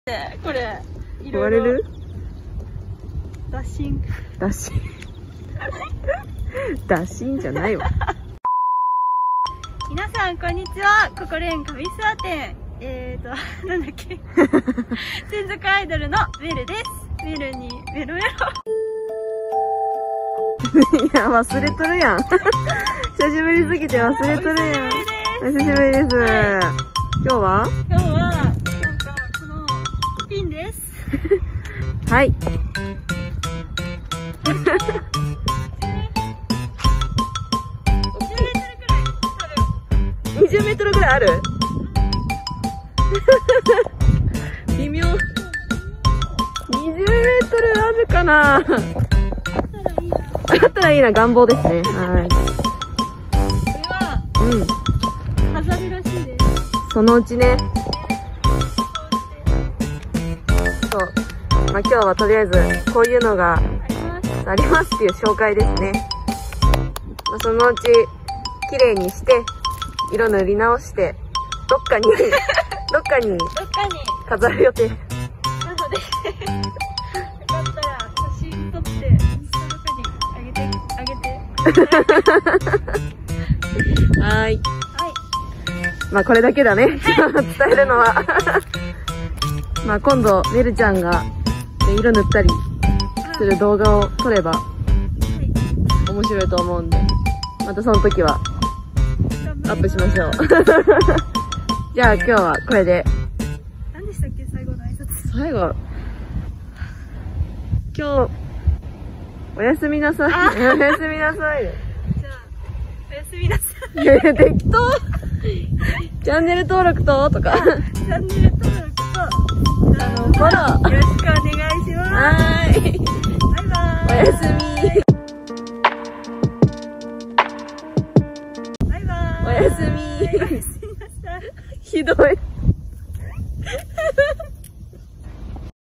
【衝撃】ココレーンに巨大ピンが⁈めるちゃん興奮!!
こんばんは！

今回は巨大ピンの紹介です!!
大きすぎてびっくりしました（笑）

めるの登場久しぶりだったので
挨拶やらキャラをすっかり忘れてましたね💦
温かい心で見てください💓

めるにめろめろ～💓